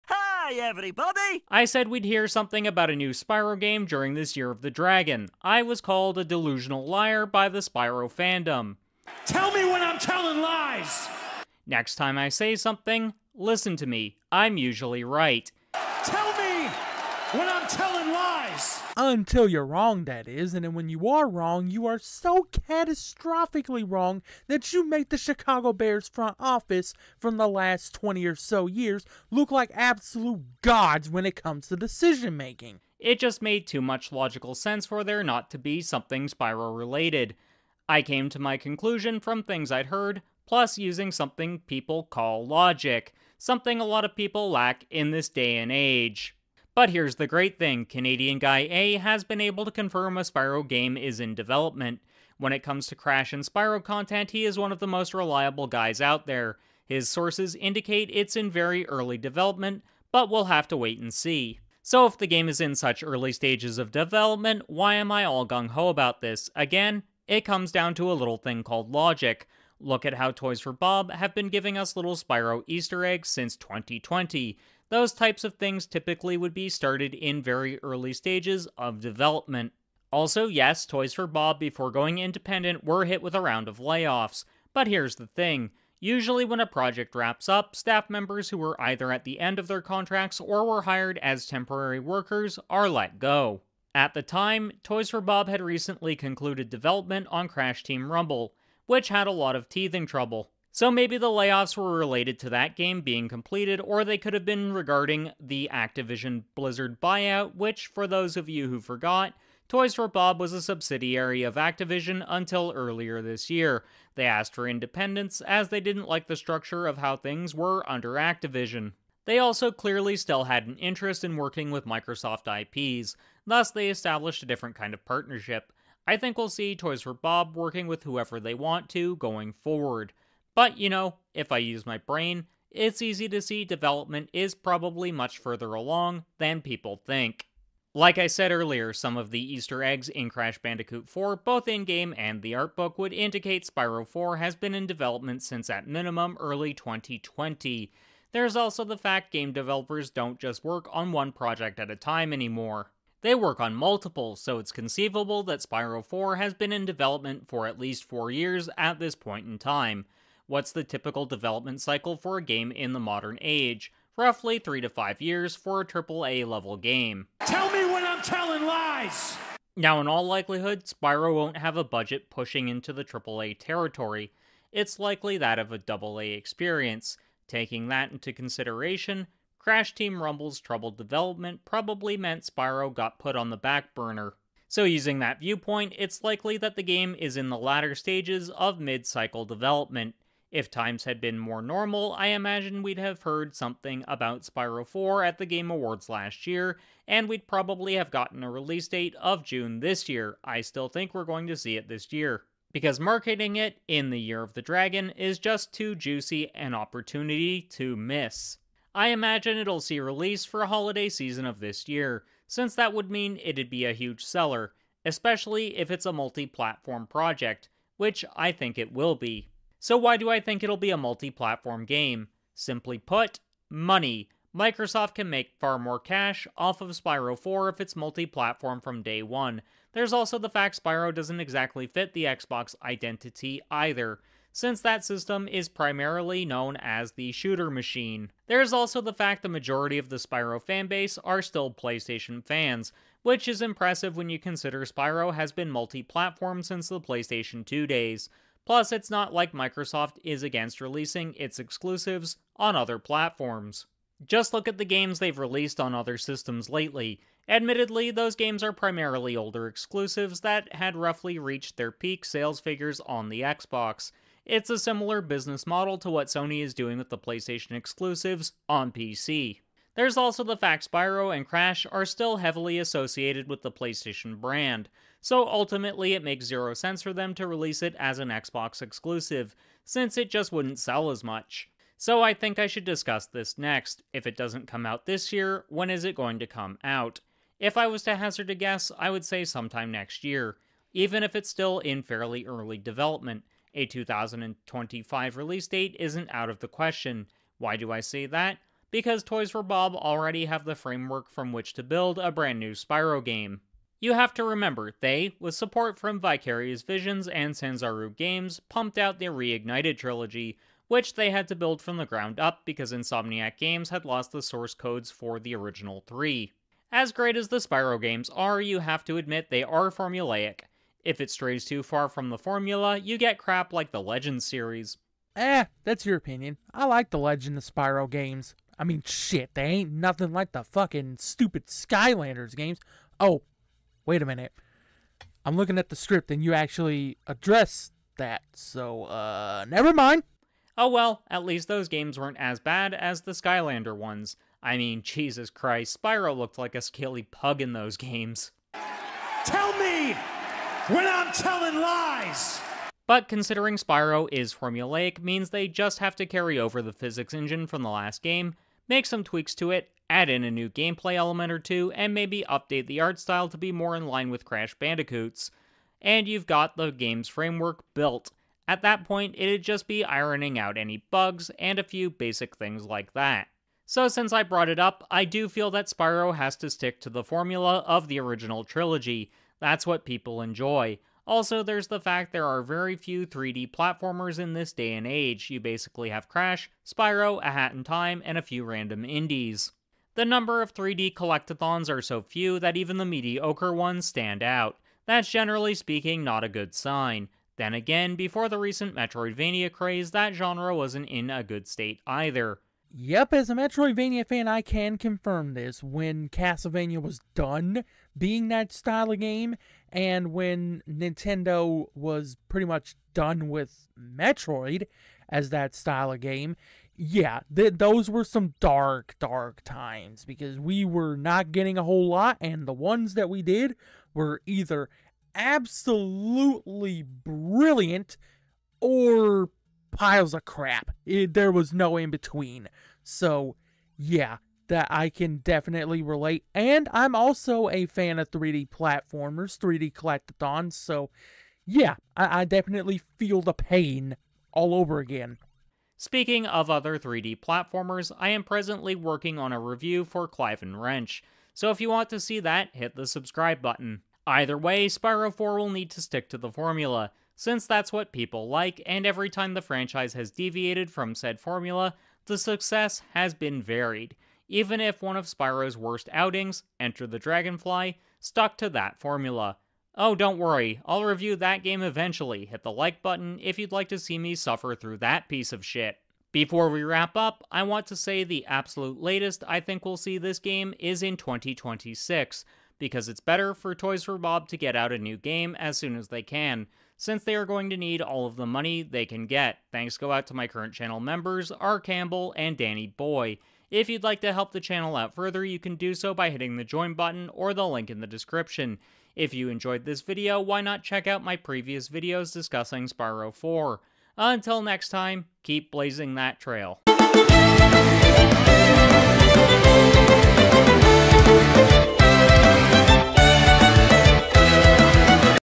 Spyro 4 Enters Development
Spyro the Dragon was one of the PlayStation 1's primary mascots. Despite there only being three exclusive games in the franchise, Spyro has remained a PlayStation icon. Now Spyro 4 has officially entered development. Canadian Guy Eh stated in one of his recent videos it's in early development, but in all likelihood it's probably much further along than people think. So what stage of development is Spyro 4 likely in?

Edited by: @TheMadBelmont
Thumbnail by: @daniboy9198